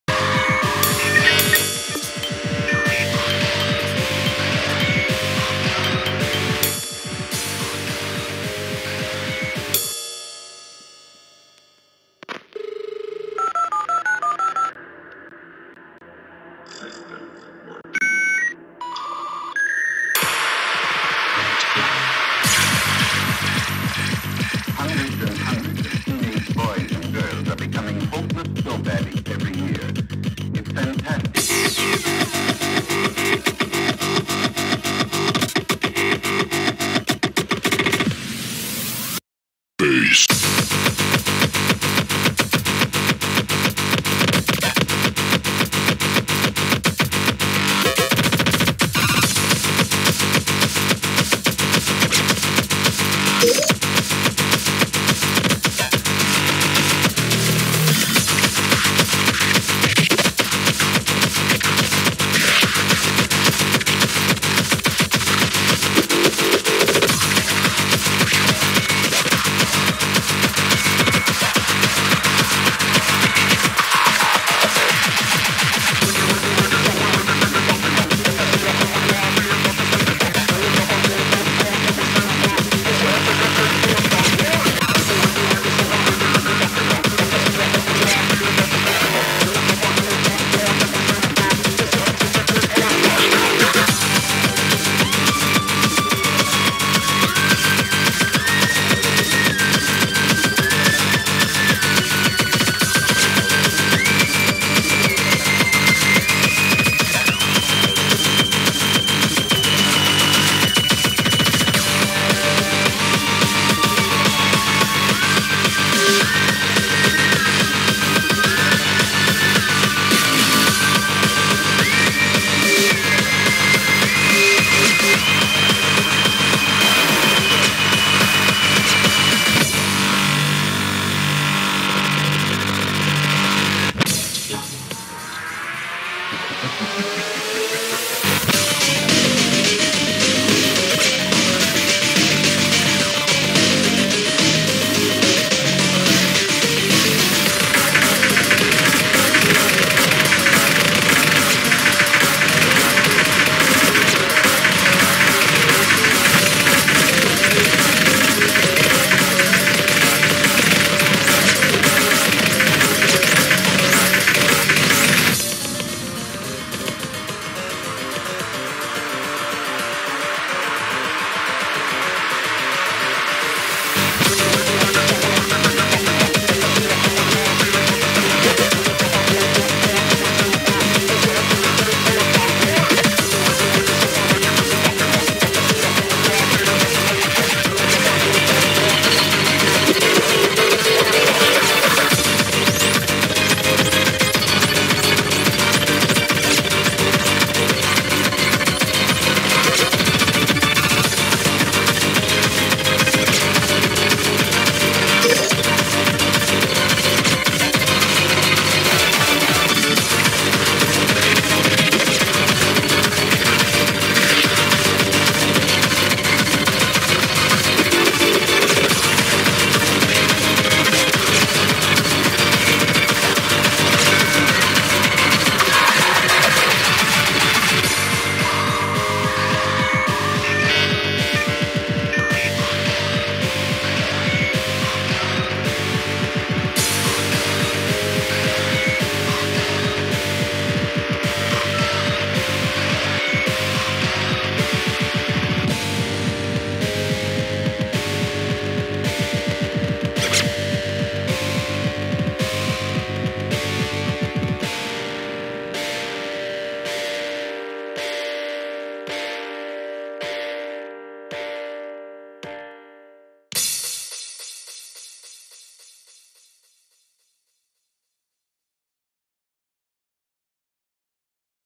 Stamina improved. Now working on sight reading!
P.S. I still suck like mad. And too bad this isn't a ranked song. Could have squeezed a little PP from this song.